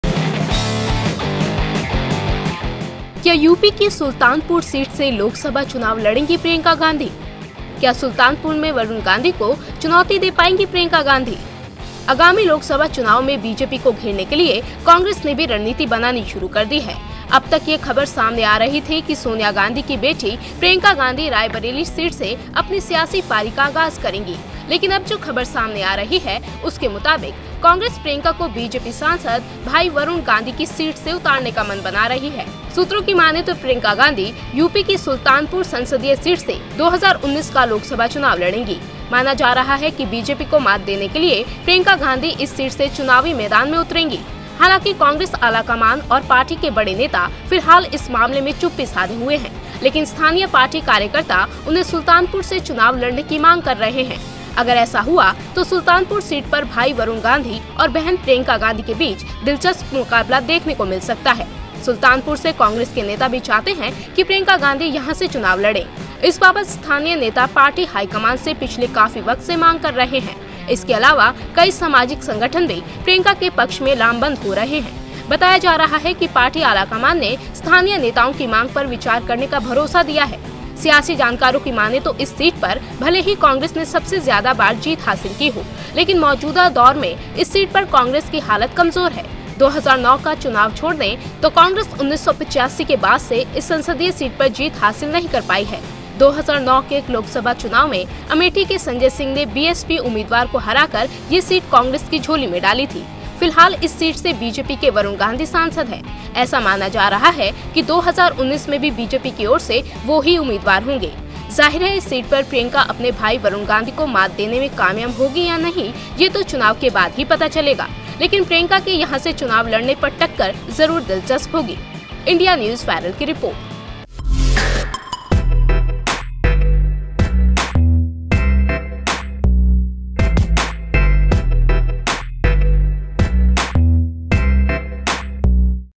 क्या यूपी की सुल्तानपुर सीट से लोकसभा चुनाव लड़ेंगी प्रियंका गांधी ? INDIA NEWS VIRAL
क्या यूपी की सुल्तानपुर सीट से लोकसभा चुनाव लड़ेंगी प्रियंका गांधी ? क्या सुल्तानपुर में वरुण गांधी को चुनौती दे पाएंगी प्रियंका गांधी ? जानने के लिए देखें रिपोर्ट